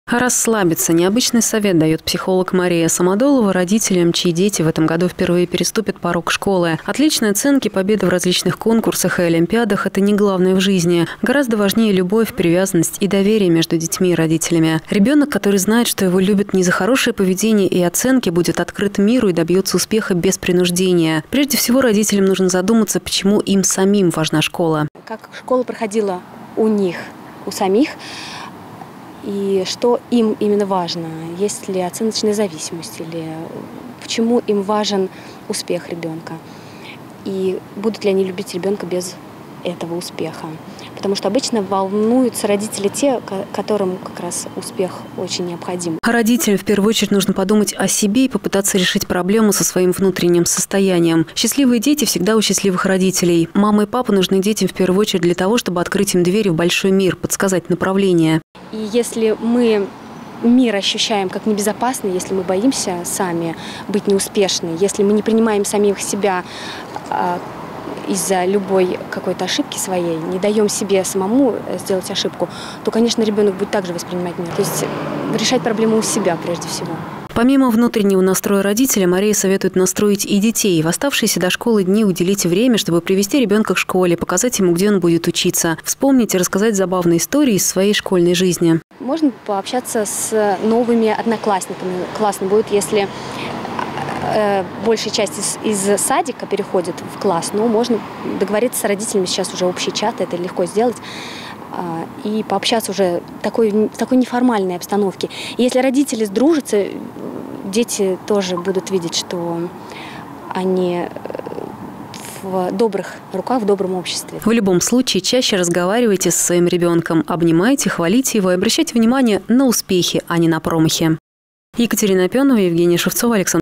Как адаптировать ребенка к школе?
Совсем скоро школы распахнут свои двери для первоклассников. Для этих детей начнется новый этап в жизни. Как сделать так, чтобы процесс привыкания к новым условиям был легким и безболезненным как для ребенка, так и для его родителей?